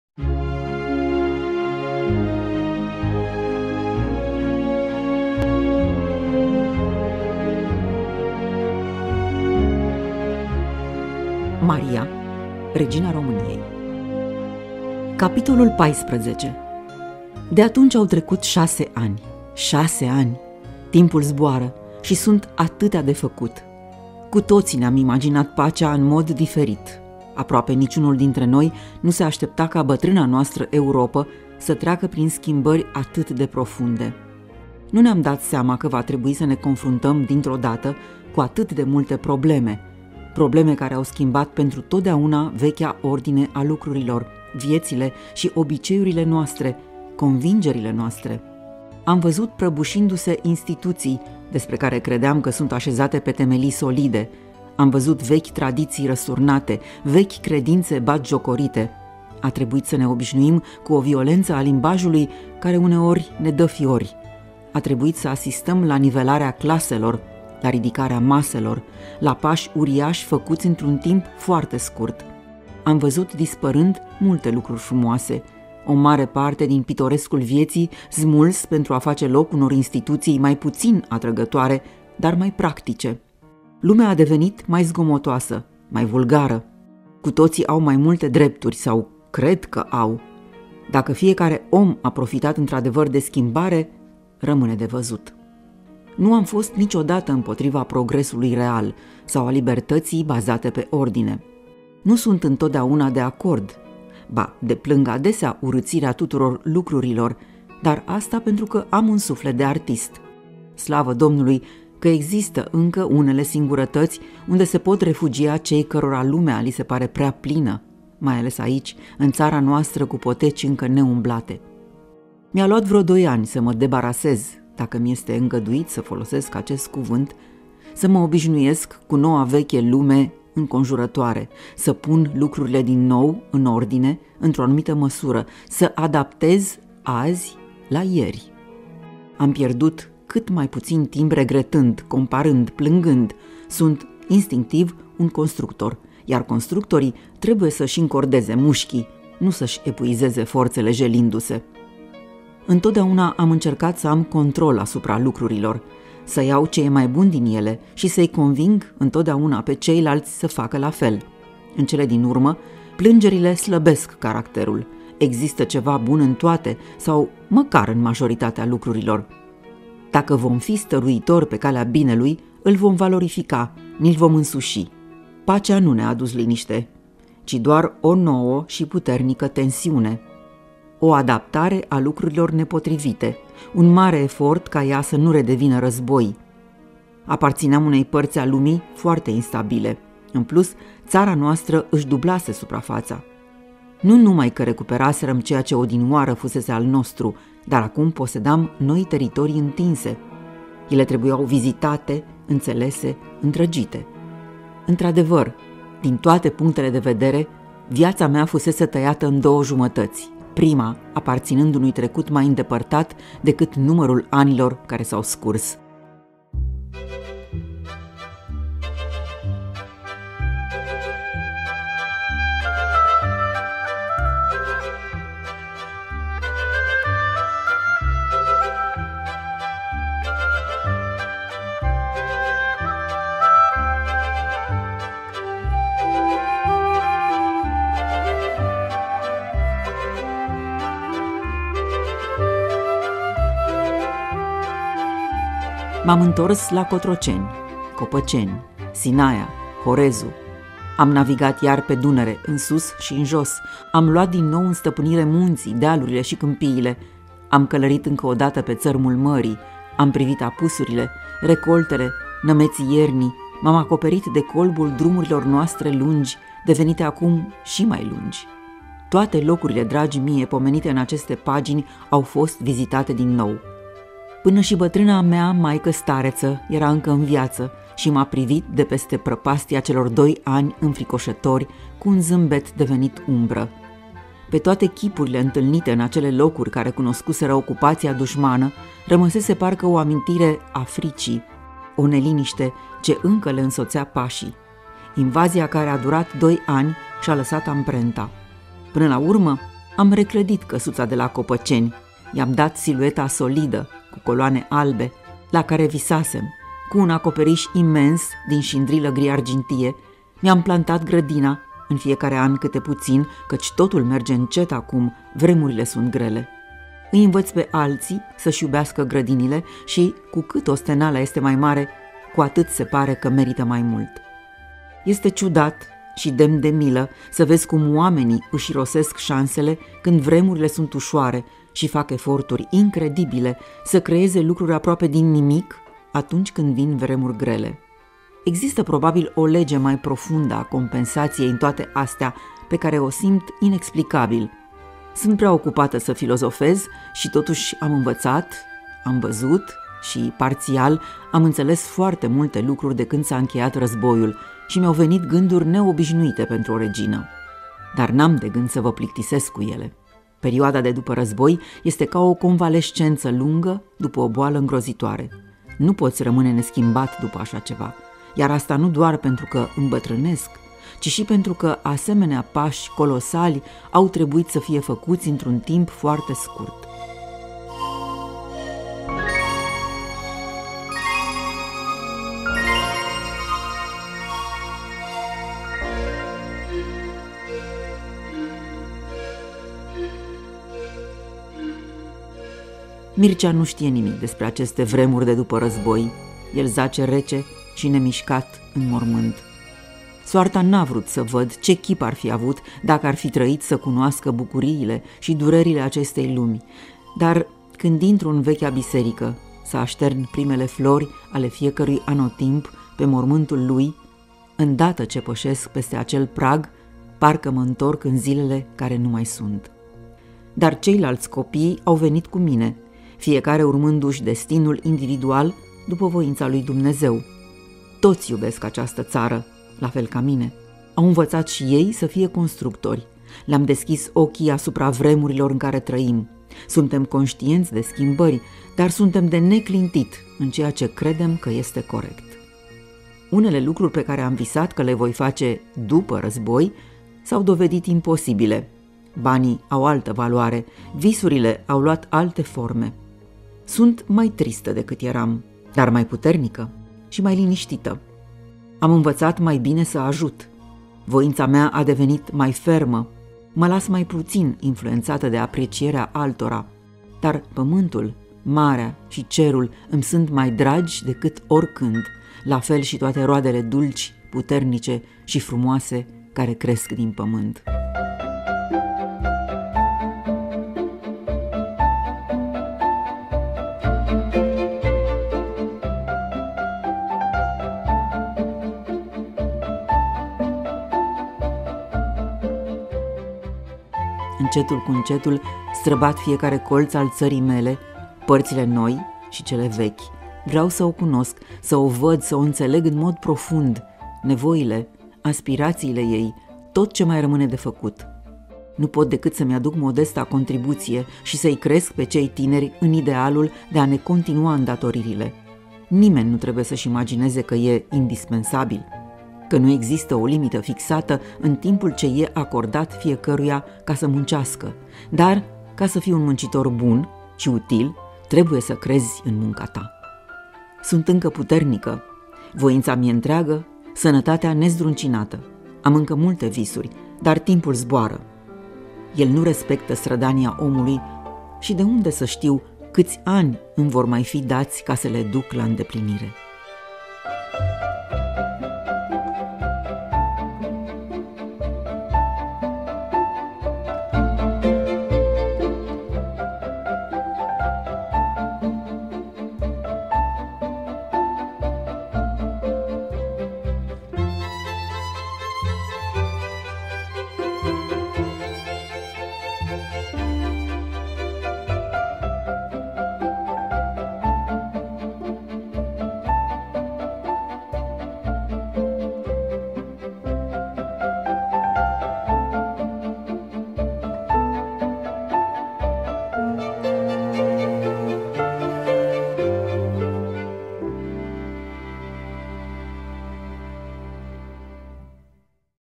REGINA MARIA - Țara pe care o iubesc / Capitolul 14
La 100 de ani de la încoronarea Regelui Ferdinand I și a Reginei Maria, Radio România Cultural a omagiat cele două mari personalități istorice ilustrând prin lecturi  epoca în care au trăit, momentul uriaș al istoriei noastre reprezentat de încoronare. De pe 10 și până pe 15 octombrie 2022,  Dana Ivanov a fost, simbolic, glasul Reginei Maria, pentru a citi ample fragmente din cartea ”Țara pe care o iubesc”, apărută la Editura Librex.